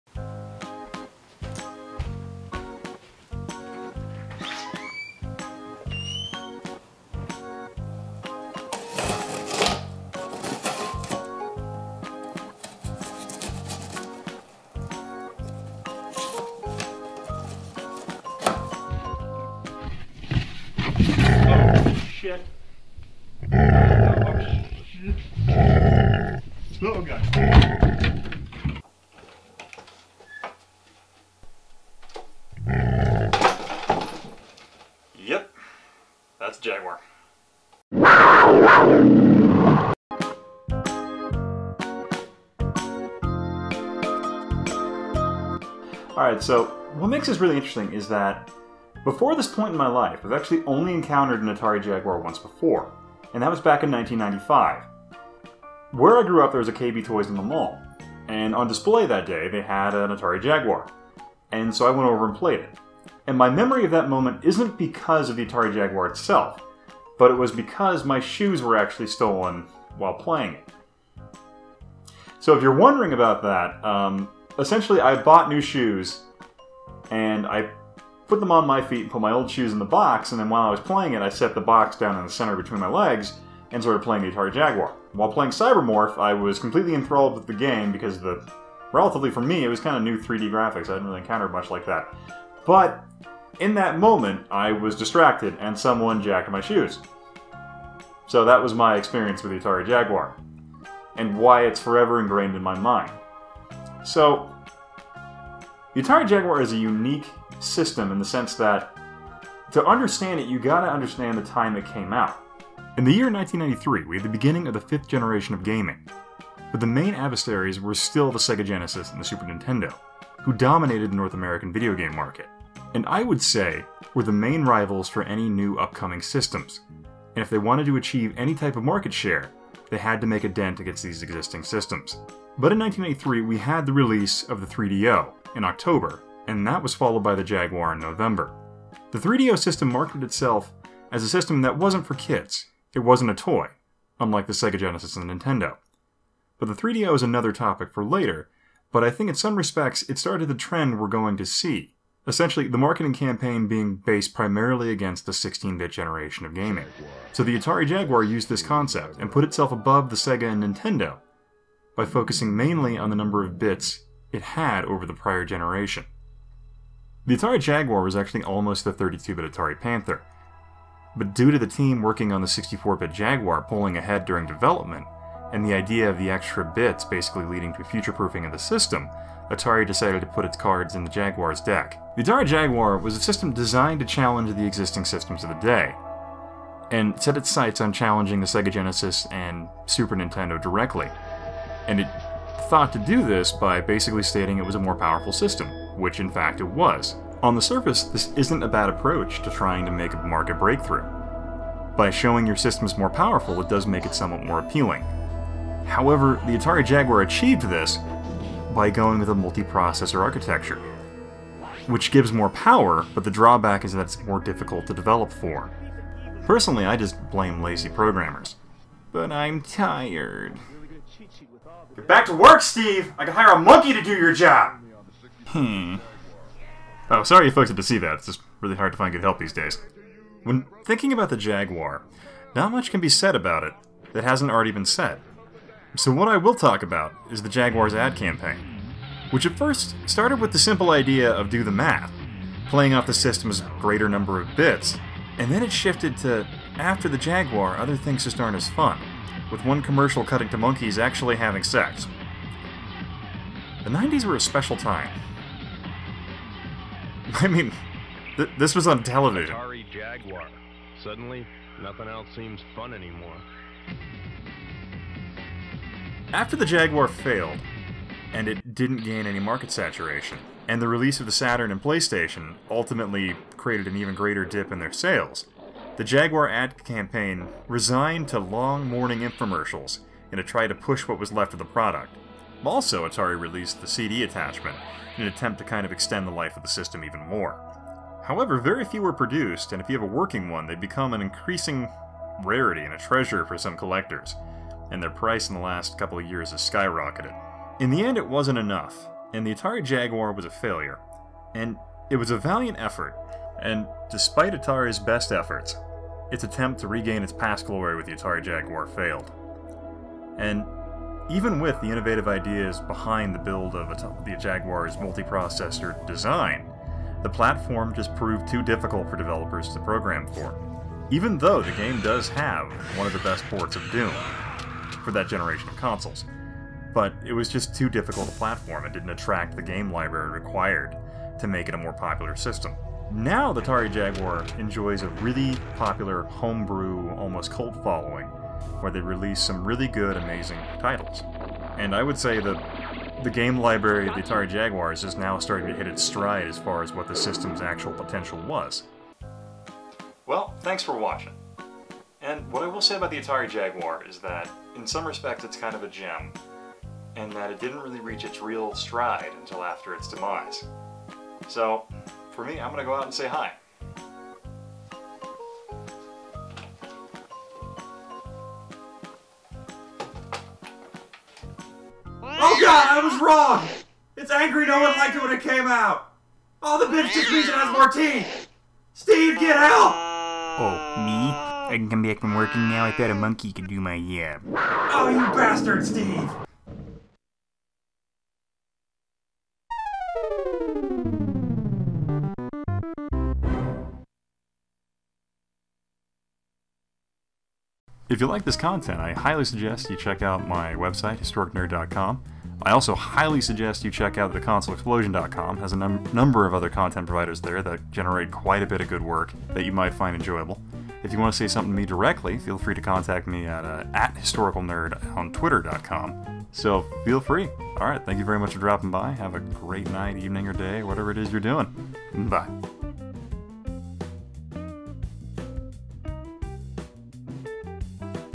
Historicnerd: ATARI JAGUAR
Join us as we dive into the most elusive of beast. The mid 90s Jaguar, a shy creature that in recent years has been hunted to near extinction by collectors, making any find outside of the internet near impossible. But what if this creature shows up at your door.